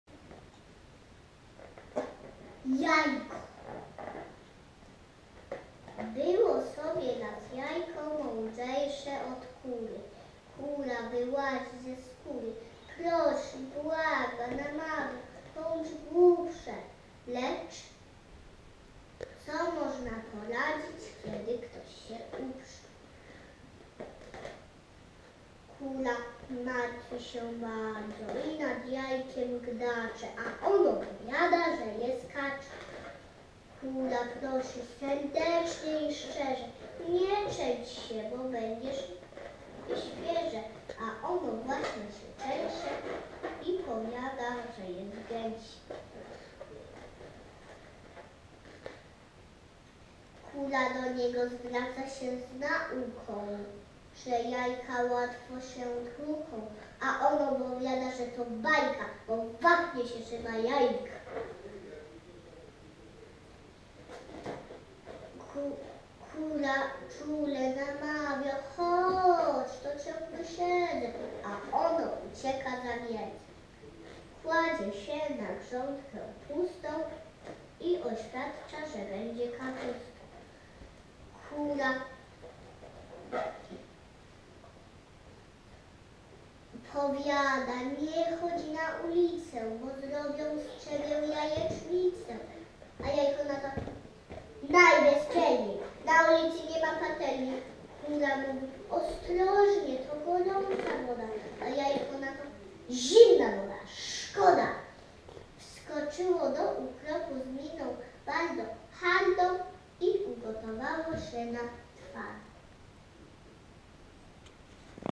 "Jajko" w Michałkowym wykonaniu
Recytacja wiersza Jana Brzechwy "Jajko"